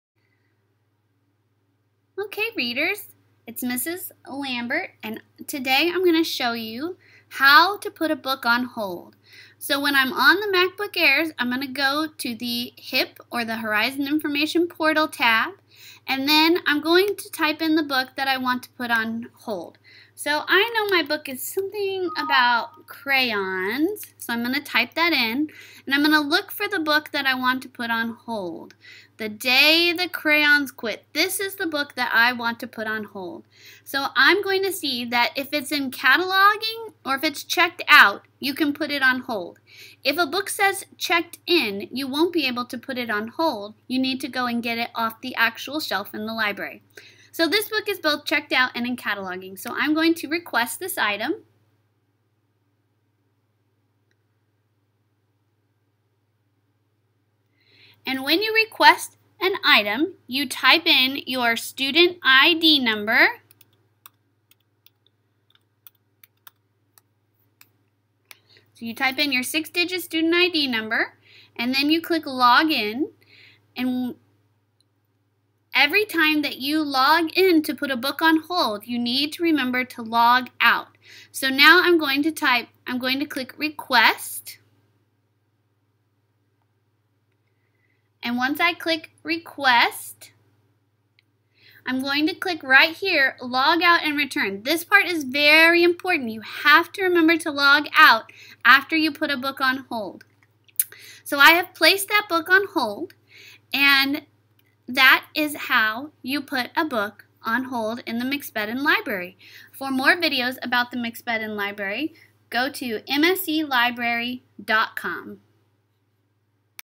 How To Put A Book On Hold
This video shows students how to put a book on hold.